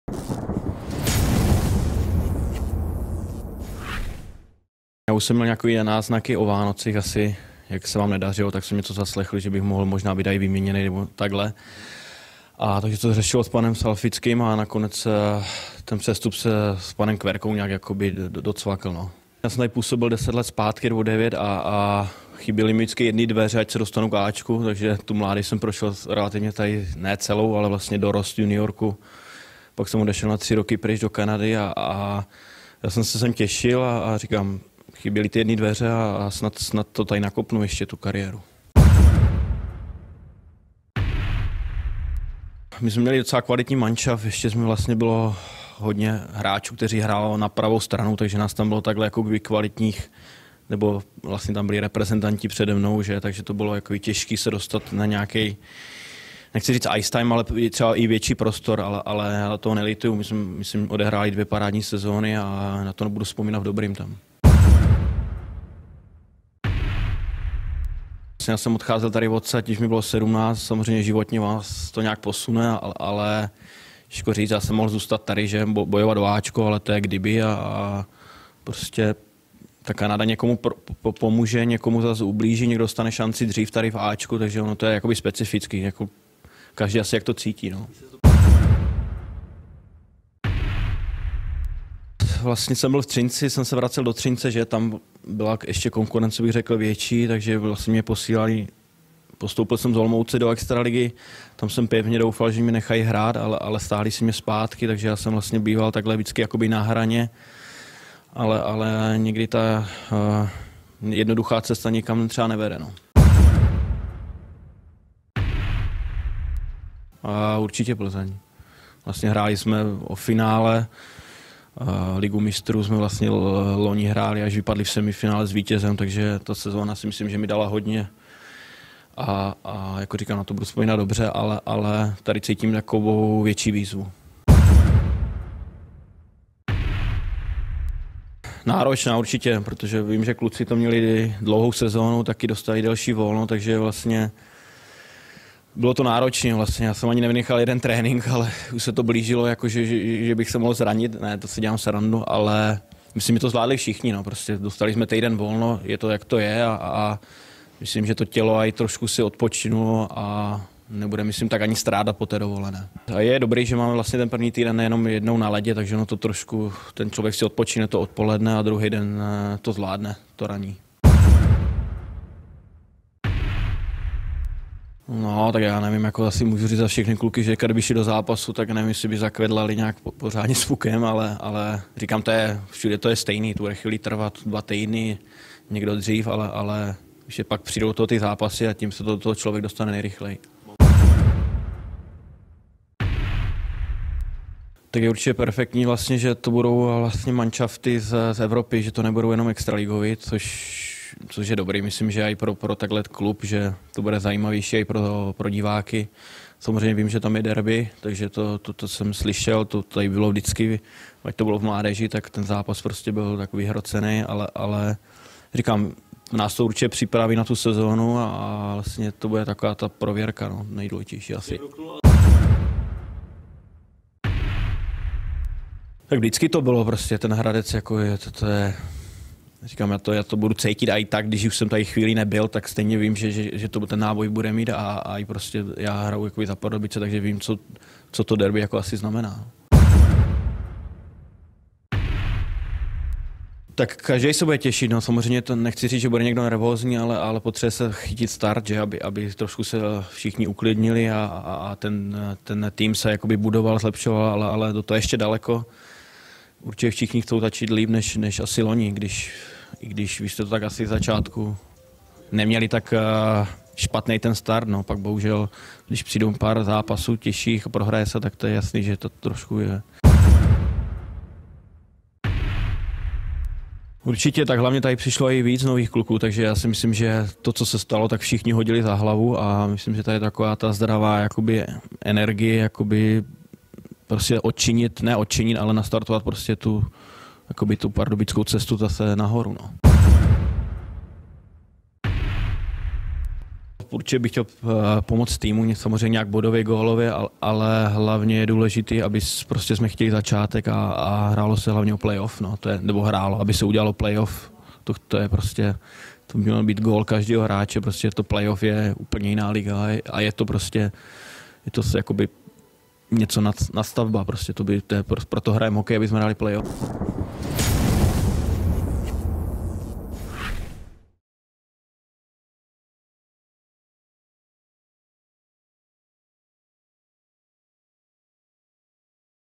Útočník DENIS KINDL: Těšil jsem se na návrat do Pardubic!
Útočník DENIS KINDL nepřišel do neznámého prostředí. V Pardubicích působil již v mládežnických letech, v sezoně 2019/2020 oblékne poprvé dres A týmu Dynama.

„První náznaky, že bych se mohl do Pardubic vrátit formou výměny, jsem zaznamenal o Vánocích, tedy v době, kdy se Pardubicích již nedařilo. Poté jsme řešili můj příchod s panem Salfickým po skončení sezony a s panem Kverkou jsme to dotáhli,“ řekl šestadvacetiletý útočník.